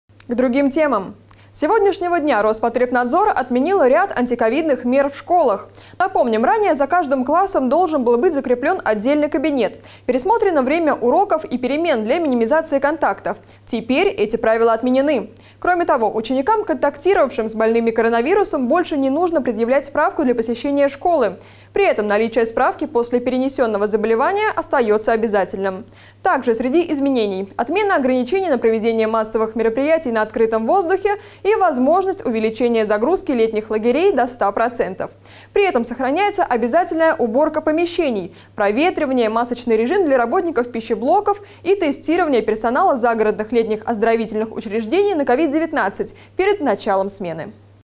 Роспотребнадзор отменил ряд антиковидных мер в школах
С сегодняшнего дня Роспотребнадзор отменил ряд антиковидных мер в школах. Ранее за каждым классом должен был быть закреплён отдельный кабинет, пересмотрено время уроков и перемен для минимизации контактов. Теперь эти правила отменены. Кроме того, ученикам, контактировавшим с больными коронавирусом, больше не нужно предъявлять справку для посещения школы. При этом наличие справки после перенесённого заболевания остаётся обязательным. Также среди изменений – отмена ограничений на проведение массовых мероприятий на открытом воздухе и возможность увеличения загрузки летних лагерей до 100%. При этом сохраняется обязательная уборка помещений, проветривание, масочный режим для работников пищеблоков и тестирование персонала загородных летних оздоровительных учреждений на COVID-19 перед началом смены. (12+)